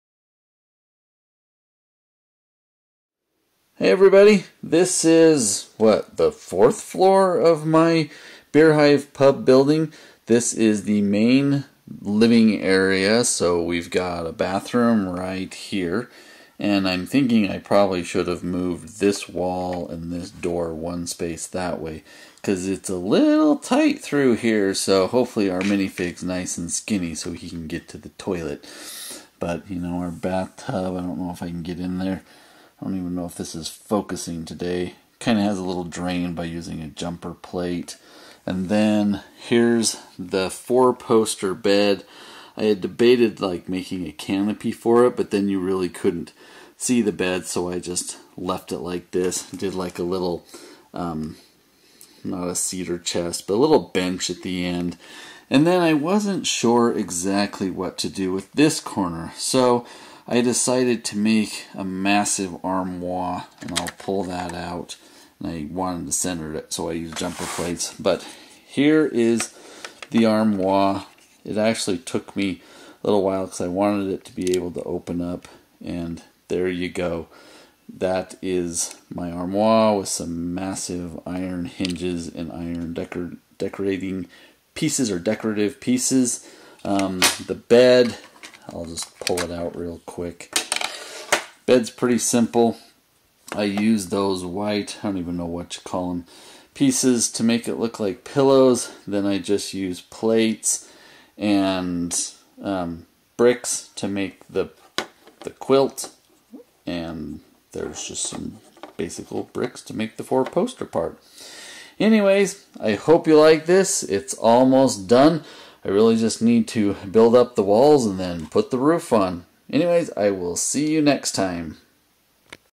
Lego Modular MOC - Beerhive Pub 4th Floor Interior
I have found that it is quite hard to get good shots of the interior once the floor is complete, so I wanted to show you the interior of the fourth and final floor of my MOC of the Beerhive Pub.  This is the main sleeping area with a bathroom.  I should have actually pushed the bathroom wall one stud toward the stairwell as it is a little tight between the bathtub and the sink, but overall it doesn't look too bad.

In the bedroom area, we have the four poster bed, two night stands, the window seat and a large armoire.  I debated making the bed a canopy, but then you couldn't see the detail, like the pillows or the comforter.  At the foot fo the bed is a bench and on each side are nightstands (as I am typing this I realize I forgot to make the lamps for the nightstands).  I actually quite like how the pillows turned out even though they don't blend perfectly with the headboard.

I debated what to put in the nook where the armoire is.  I thought of putting a chair with a bookcase, or a treadmill, but since the room doesn't have a closet, I went with the armoire.  The armoire was a little tricky as I wanted to be able to open the doors and working out a hinge that would sit flush with the armoire required a SOTS technique.  In order to achieve the correct dimension, I had to use a lantern piece on the second stud back, with a single plate on the side stud, with a since plate with a clip on top to hold the rod on the door.  It does create a small gap in the front because you cannot cover up the last stud on the main part of the armoire, but by using black for the hinges it hides fairly well.